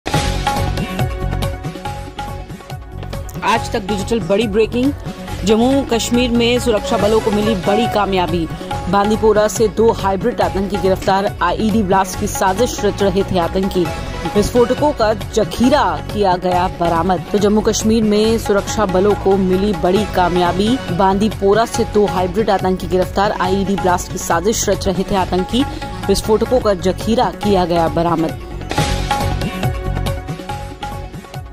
Breaking News: J&K में सुरक्षाबलों को मिली बड़ी कामयाबी, दो हाइब्रिड आतंकियों को किया गिरफ्तार
J&K में सुरक्षाबलों को बड़ी कामयाबी मिली है. सुरक्षाबलों ने बांदीपोरा से दो हाइब्रिड आतंकियों को गिरफ्तार किया है.आतंकी IED ब्लास्ट की साजिश रच रहे थे.

Security forces have got great success in J&K. Security forces have arrested two hybrid terrorists from Bandipora. The terrorists were plotting the IED blast.